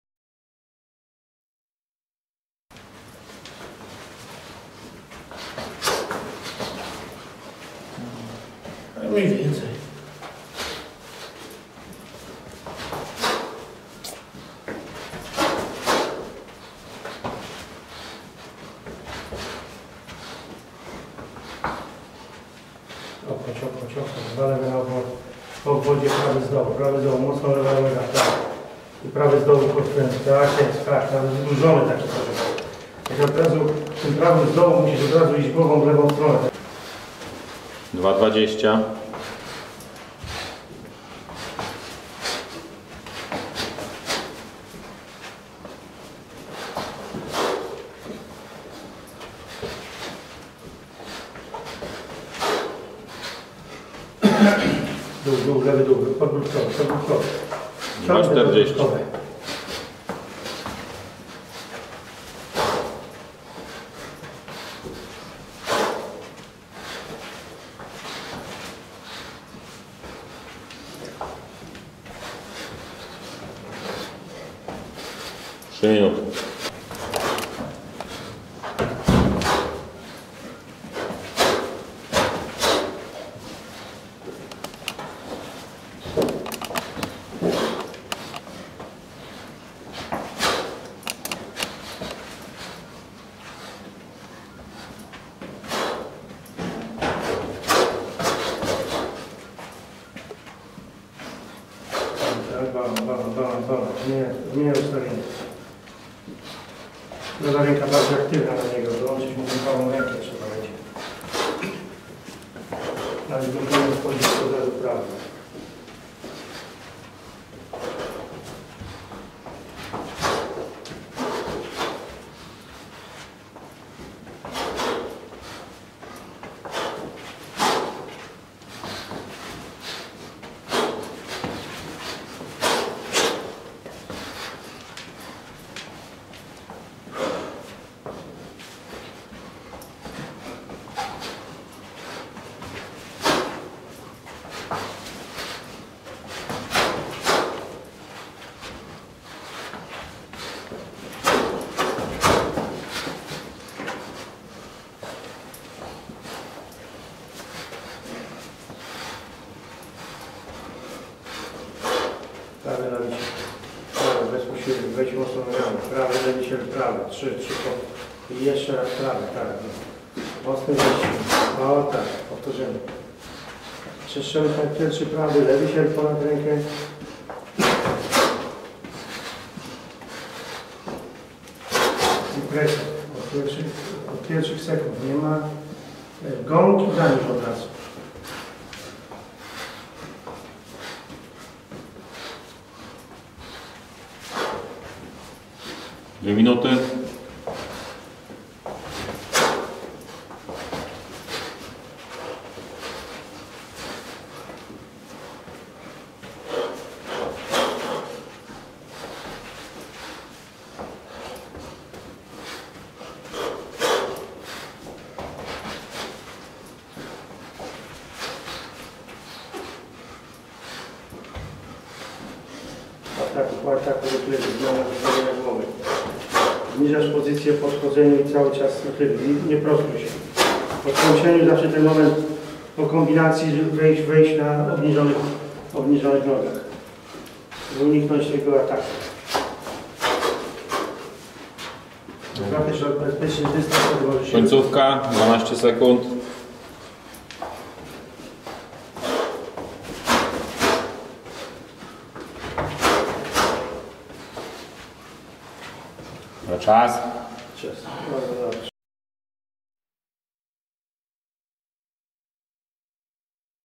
Wilczewski-DeGale - trening "Wilka" na trzy dni przed walką
- Największy portal bokserski w Polsce przedstawia przygotowania Piotra Wilczewskiego z Andrzejem Gmitrukiem, przed walką w obronie pasa EBU z Jamesem DeGale'em.